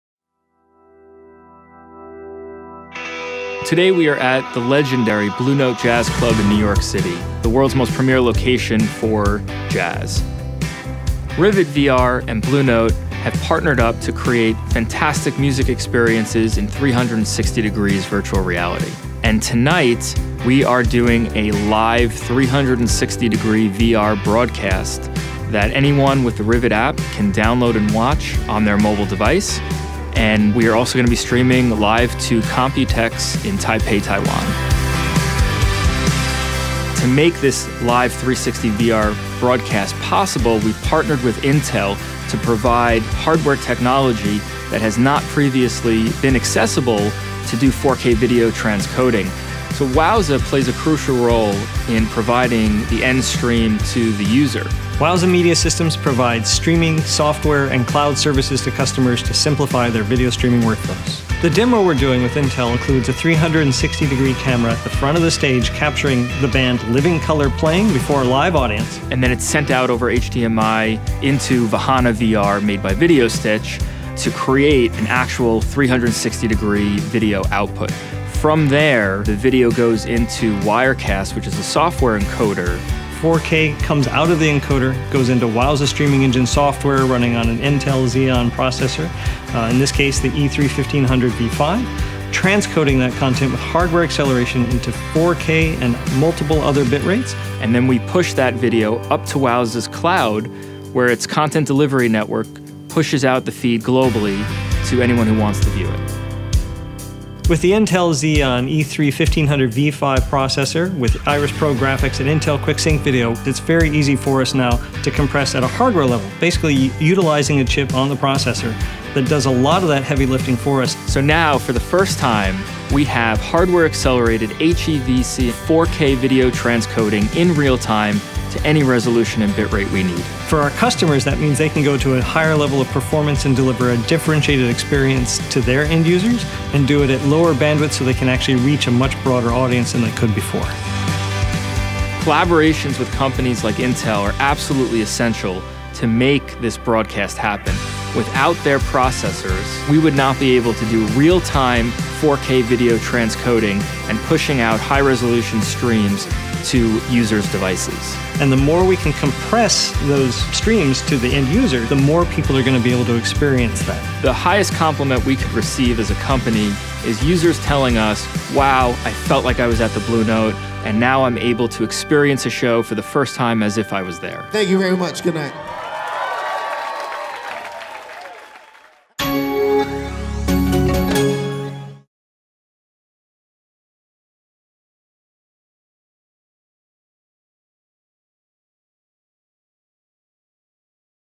Intel, Wowza, and Rivet Deliver Immersive Video Experiences | Intel Business
Intel, Wowza Media Systems, and Rivet Media have teamed up to deliver real-time streaming 4k, 360 degree virtual reality video to multiple devices by leveraging the hardware-assisted graphics capability of the new Intel® Xeon® processor E3-1500 v5.